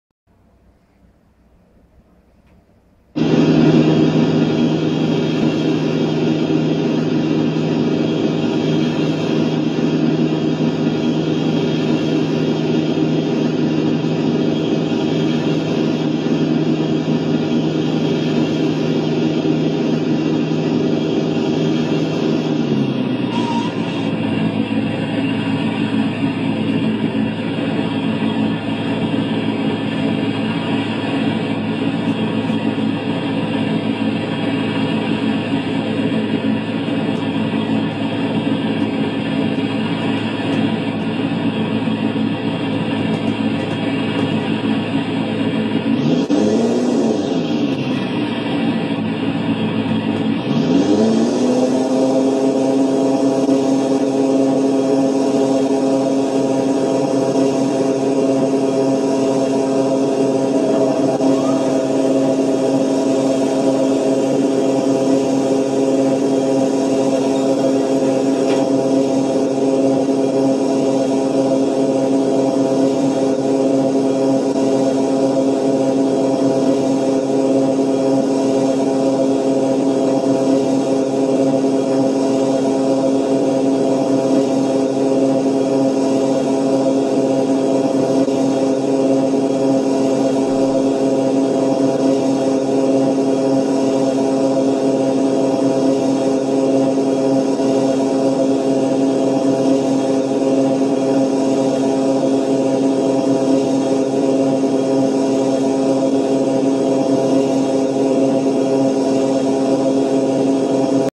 Silvão Batéra V - (LANTERNA VERDE) Pouso C-130 porta aviões, câmera lado dos turboélice allisonT56.
- (LANTERNA VERDE) Pouso C-130 porta aviões, câmera lado dos turboélice allisonT56.